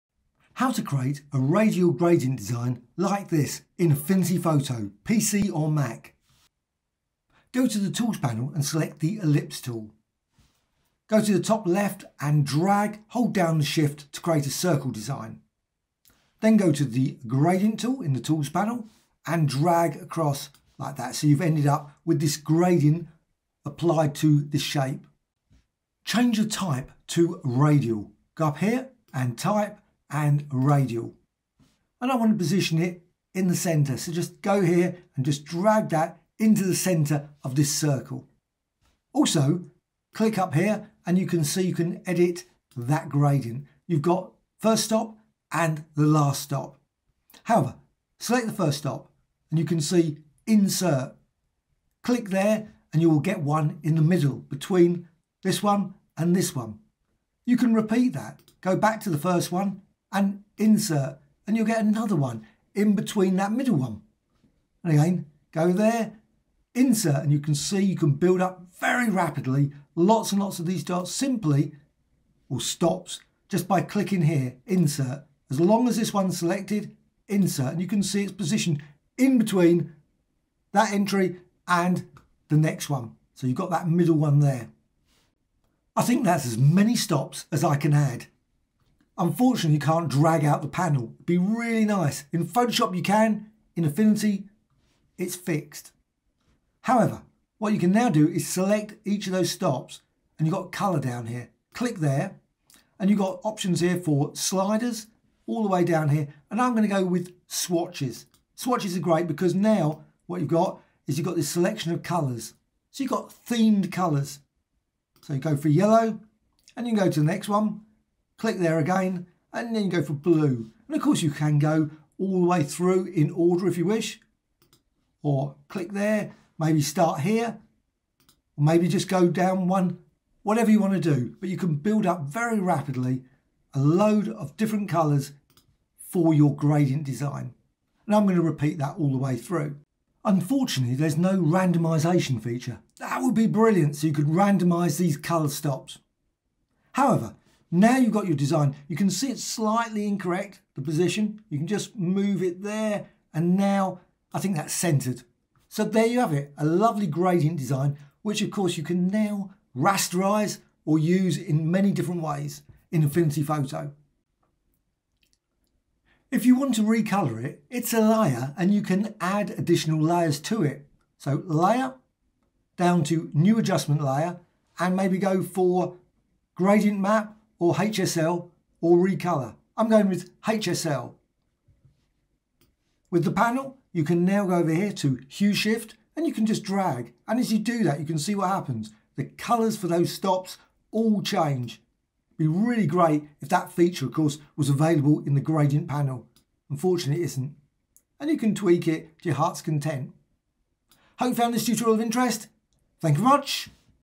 Affinity Photo : Radial Gradient Color Creation How To Tutorial
How to create a quick multi-colored radial gradient in Affinity Photo

0:00 Start of the video
0:20 Gradient
0:40 Gradient stops
1:45 Setup colors
2:50 Adjustments